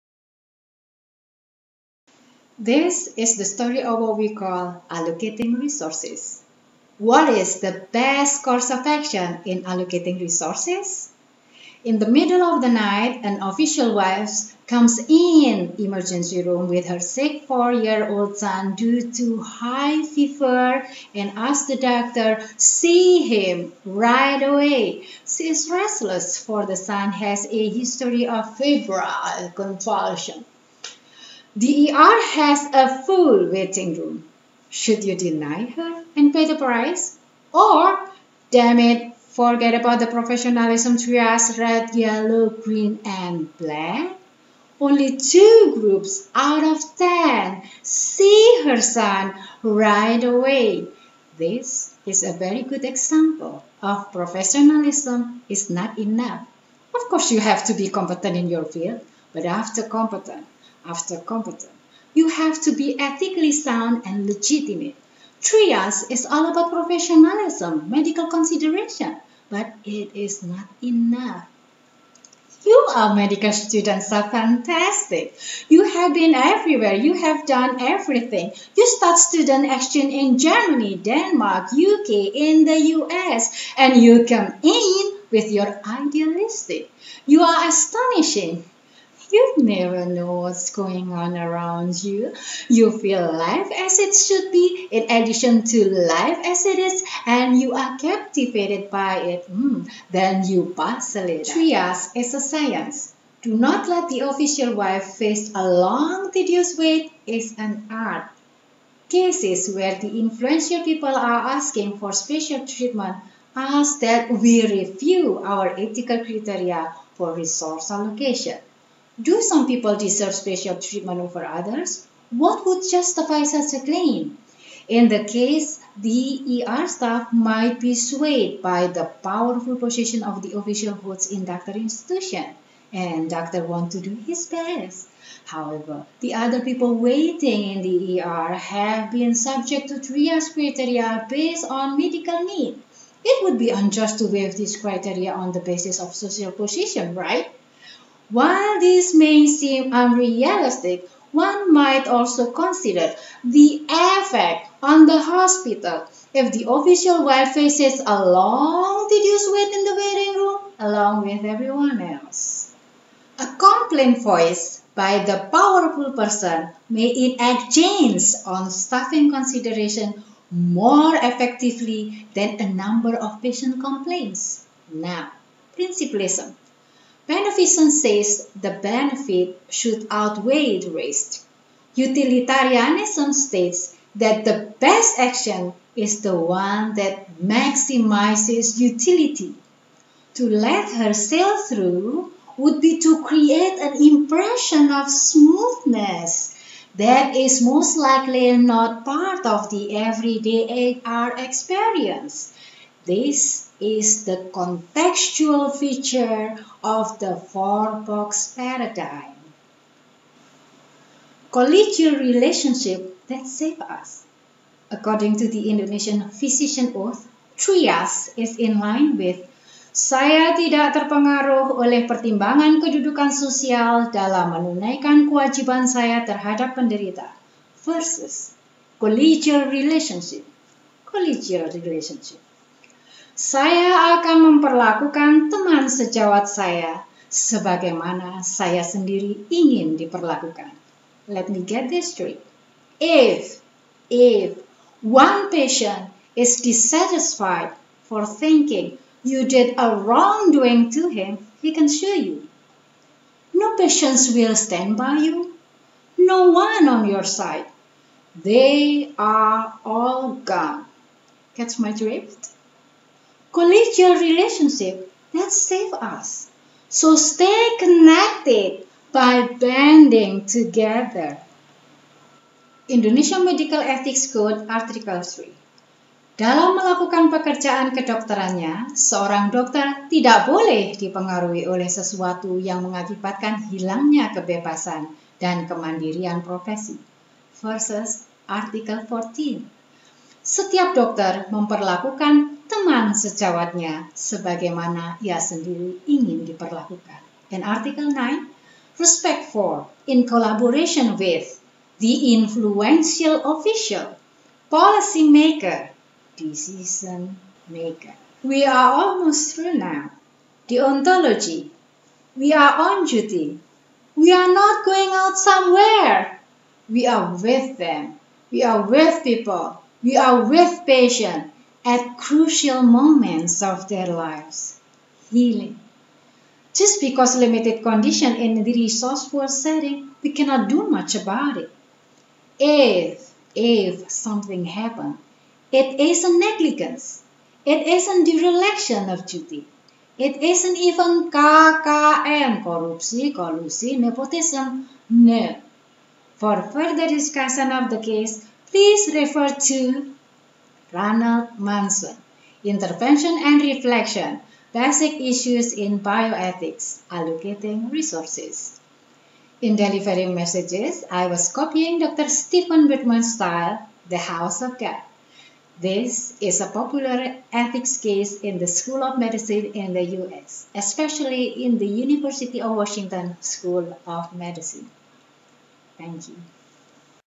Medical Ethics - Alloc
This is the story of what we call Allocating Resources. What is the best course of action in allocating resources?

In the middle of the night, an official wife comes in ER with her sick 4-yo son due to high fever and asks the doctor see him right away. She is restless for the son has a history of febrile convulsion. The ER has a full waiting room.

Her husband is a physician at no practice for 10 years due to his high post and now serves as a mayor. Son's diagnosis: Tonsillitis. There are 16 patients with Triage Card:1  red, 9 yellow, 5 green, and 1 black. The patient in red code is undergoing referral from type C to type B hospital.

Should you deny her and pay the price? Or, damn it forget about the professionalism triage-red-yellow-green-black?